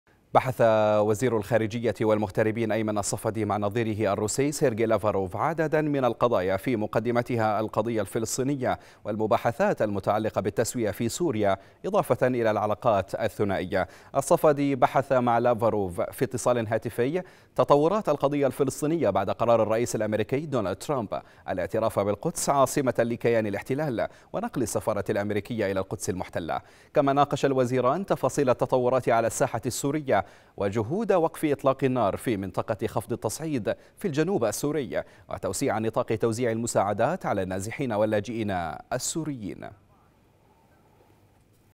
الصفدي يبحث مع لافروف عدد من القضايا - (21-12-2017)
بحث وزيرُ الخارجية والمغتربين أيمن الصفدي مع نظيرهِ الروسي سيرغي لافروف عددًا من القضايا، في مقدمتِها القضيةُ الفلسطينية، والمباحثاتُ المتعلقةُ بالتسويةِ في سورية، إضافةً إلى العلاقاتِ الثنائية.
الصفدي بحث مع لافروف في اتصالٍ هاتفي تطورات ِالقضيةِ الفلسطينية بعد قرارِ الرئيسِ الأمريكي دونالد ترامب الاعتراف بالقدسِ عاصمةً لكيانِ الاحتلال ونقلِ السفارةِ الأمريكية إلى القدس المحتلة.